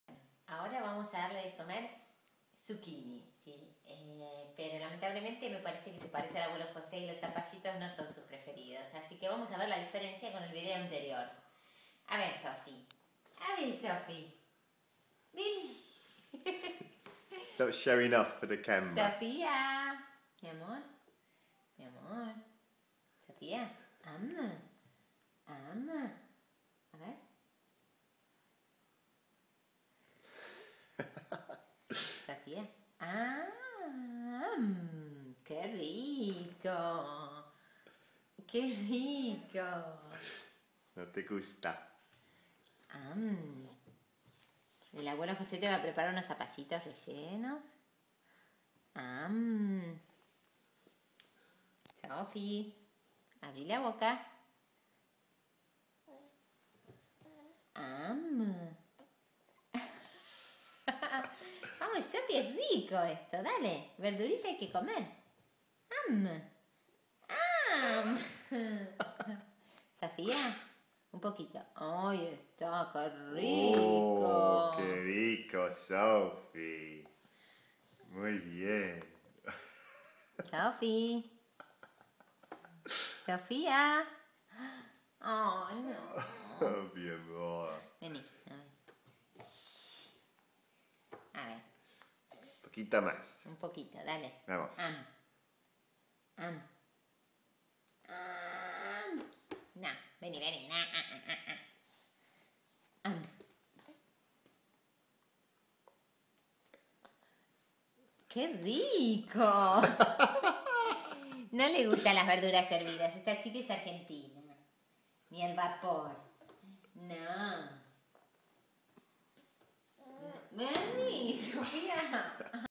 Zapallitos
Sofi no le gusta todo su comida